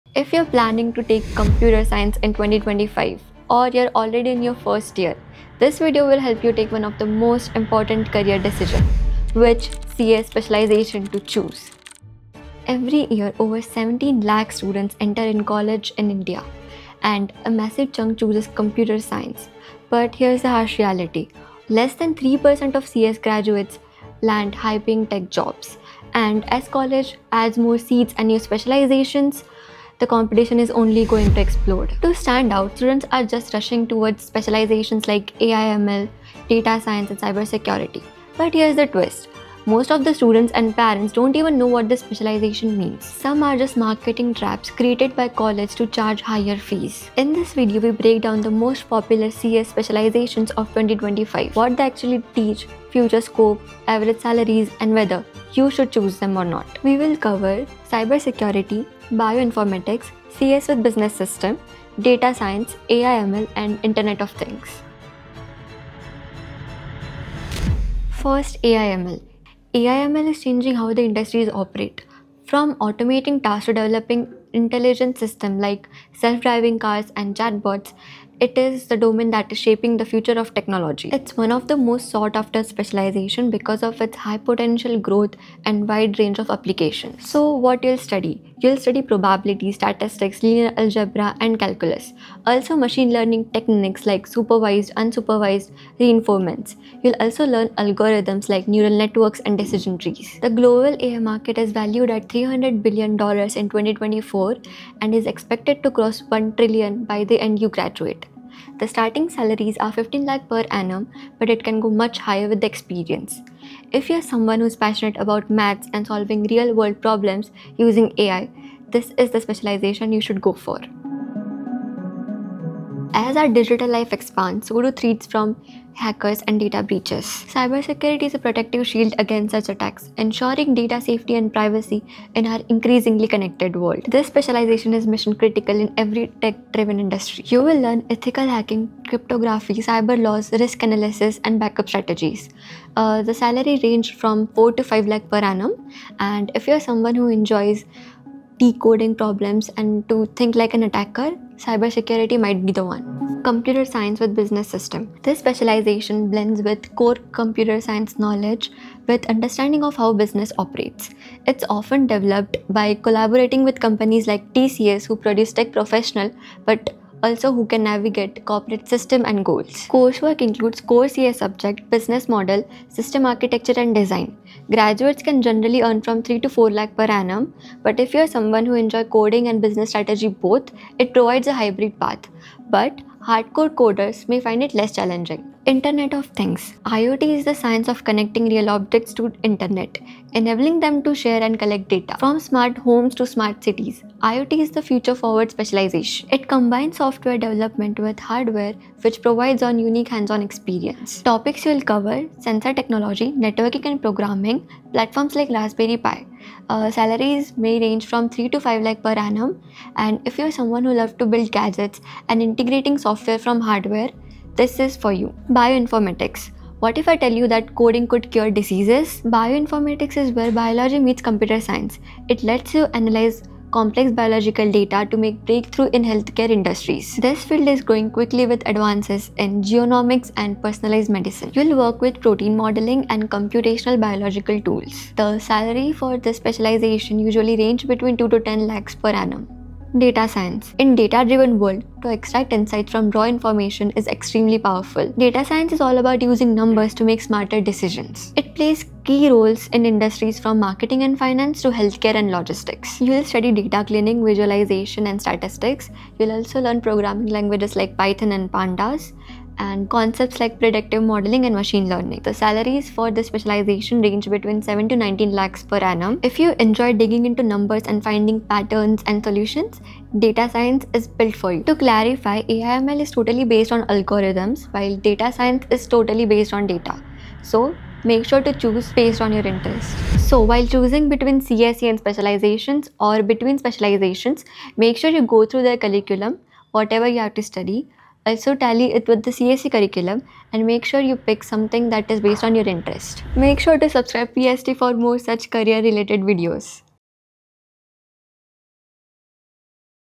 Watch This Before Picking Your CS Specialization !
Thousands of students enter CS every year, only a few land top tech jobs. This video breaks down the top specializations in 2025 (AI/ML, Data Science, Cybersecurity, IoT, Bioinformatics, and CS + Business Systems). Learn what you’ll actually study, the real-world job scope, salary ranges, and what’s hype vs reality. If you’re choosing your CS path this year, this is a must-watch.
Join the official PST Telegram Group for the latest updates, PAT preparation and doubt resolution